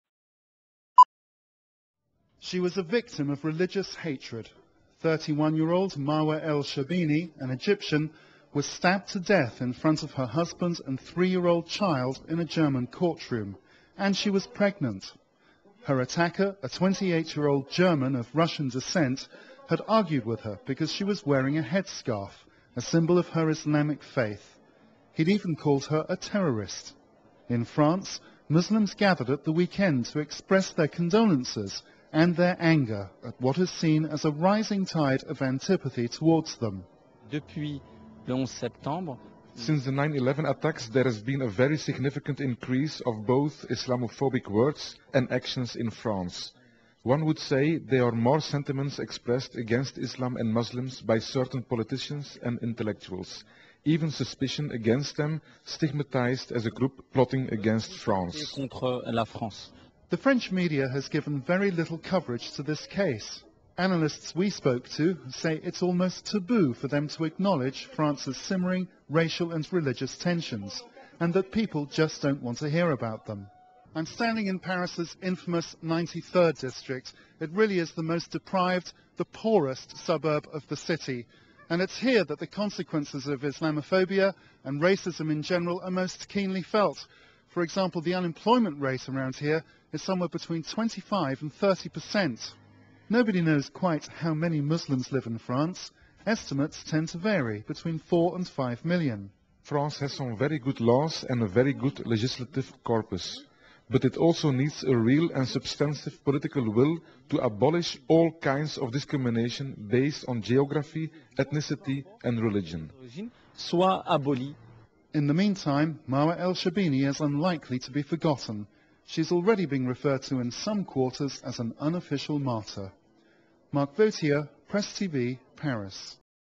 French Muslims voice anger over rising Islamophobia
Press TV, Paris